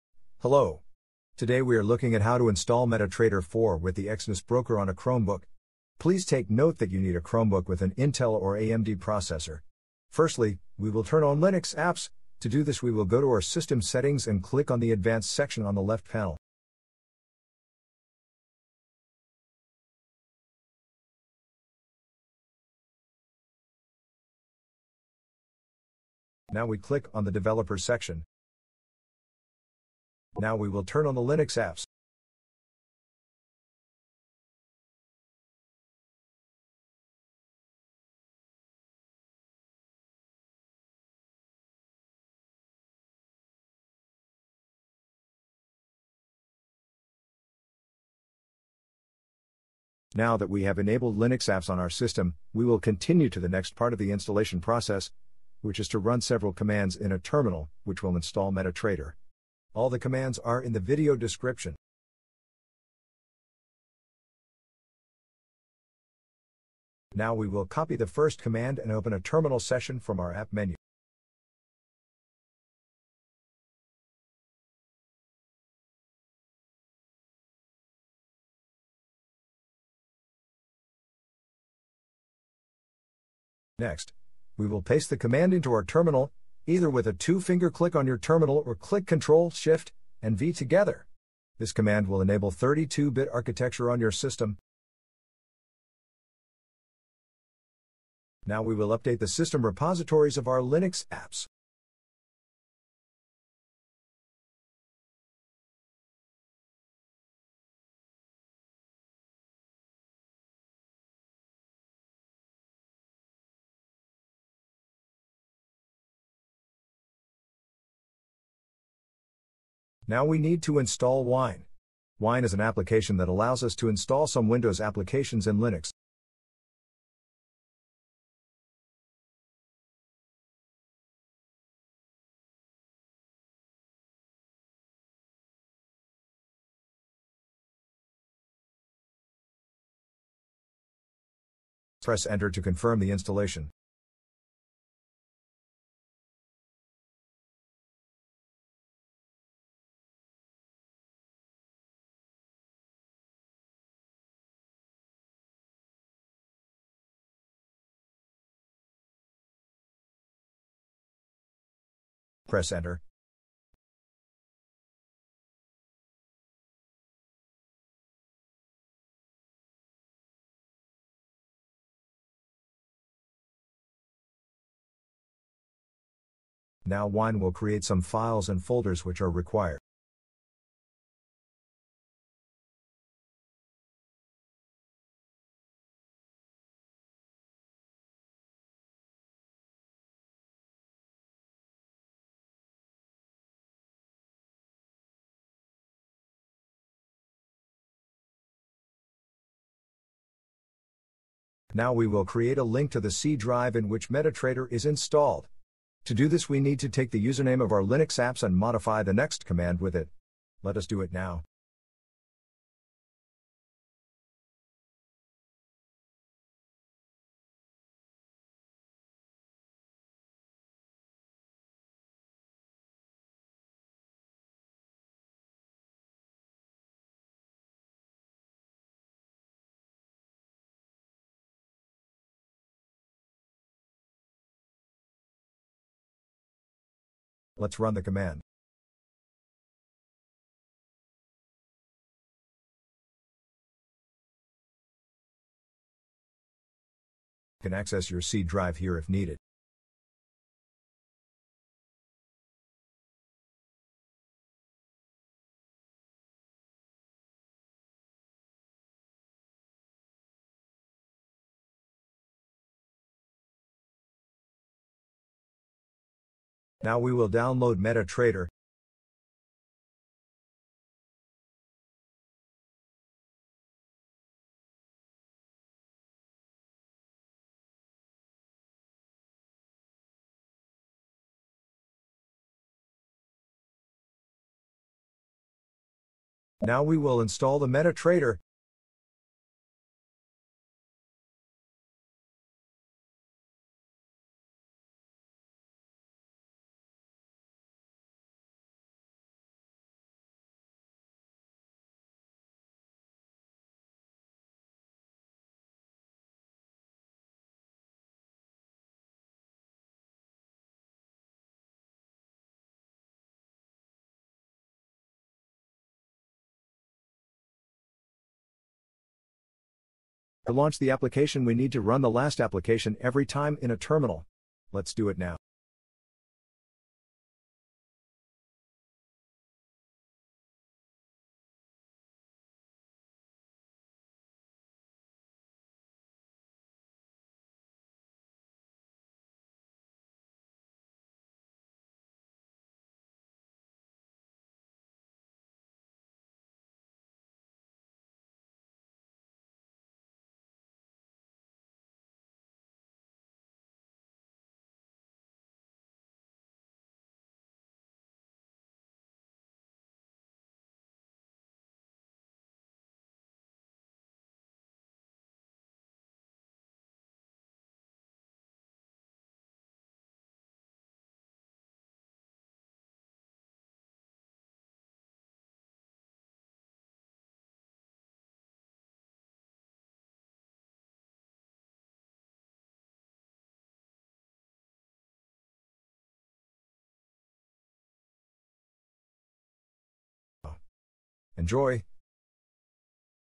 How to install MetaTrader 4 with the EXNESS Broker on a Chromebook
In this video, we are looking at how to install MetaTrader 4 with the EXNESS Broker on a Chromebook.